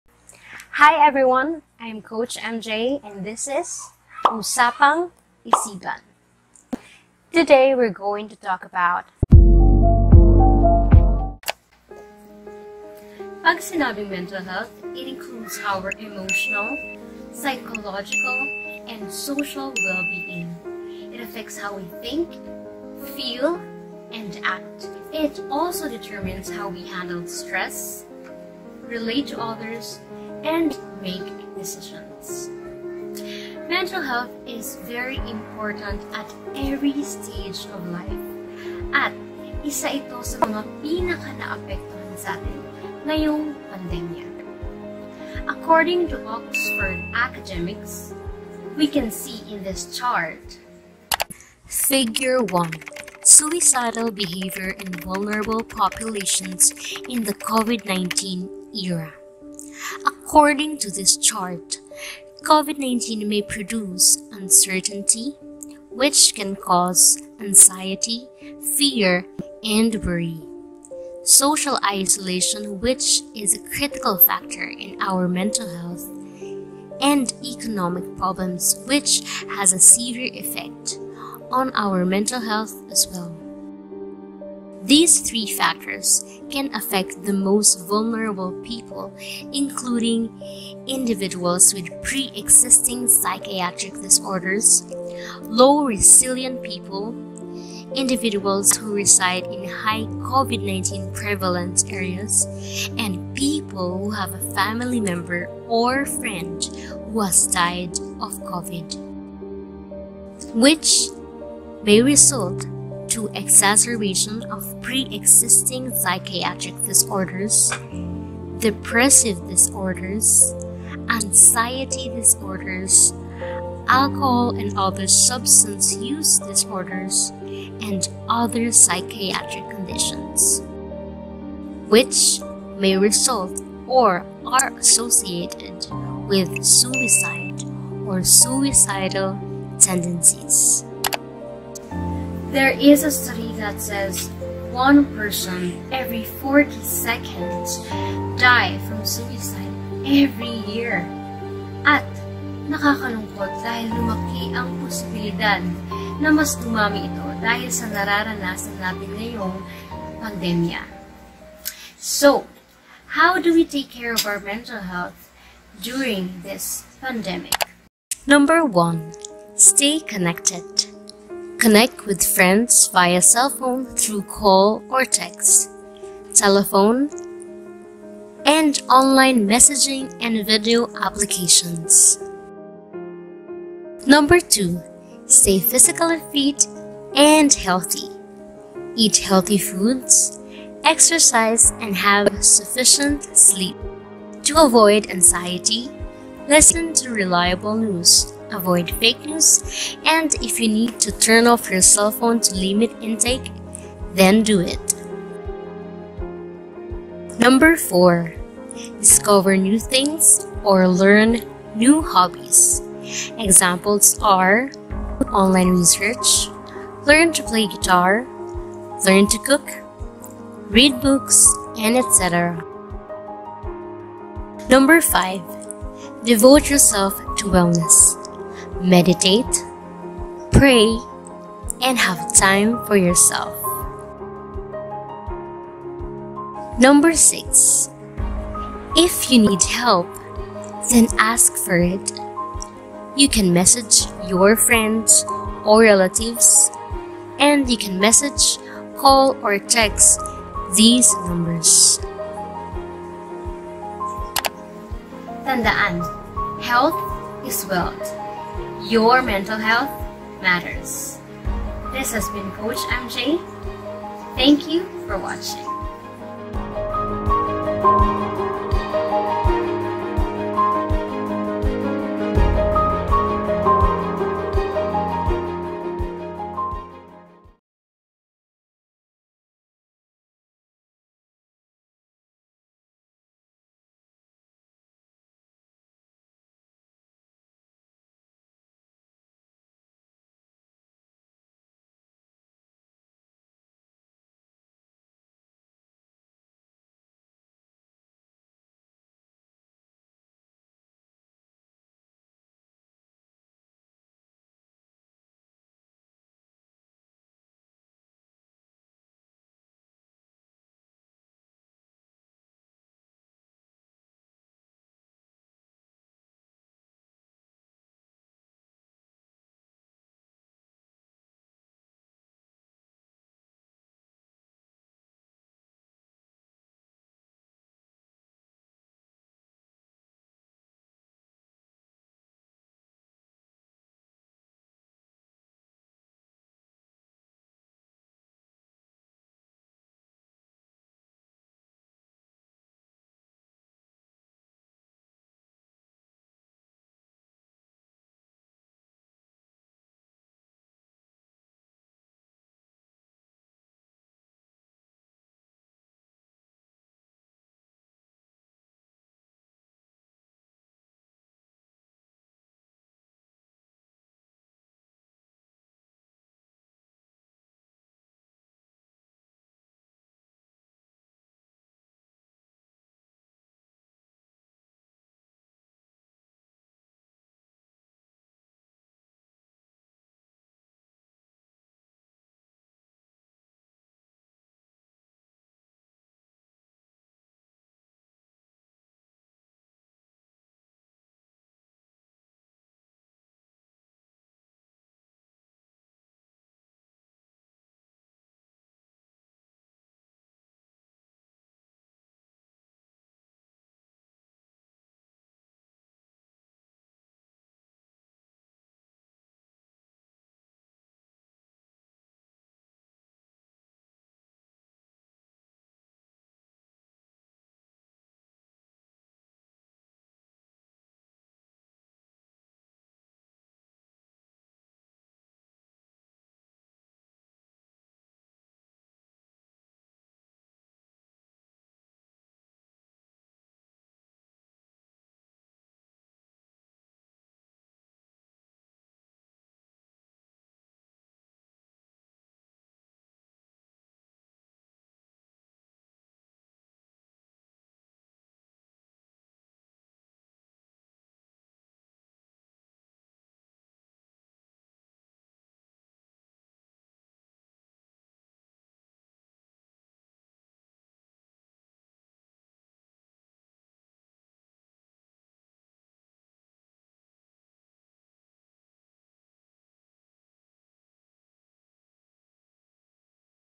What is Mental Health?
Is going to a Psychologist or Psychiatrist means "You are crazy?"

Many countries including Philippines is now in need to break this stigma.

What you are going through doesn't define you but mold you. There is nothing to be ashamed of having a Mental Illness.

Let's continue to increase the awareness of the importance of Mental Health specially this pandemic.

Here are some tips how to stay Mentally healthy.

Don't forget if you feel and think you need help.

Ask for it.

You are not alone. Do not hide.